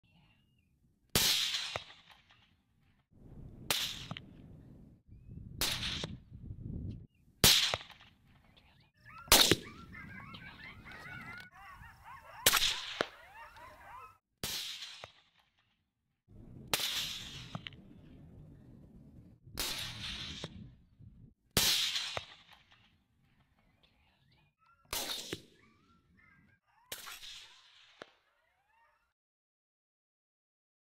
Coyote Dirt Naps | Vol 2
Coyote Hunting With The @ICOtecGameCalls Outlaw Predator Caller, @gowilsoncombat Tactical Hunter 6.5 Creedmoor, @lehighdefense 130 Grain Controlled Chaos Bullet.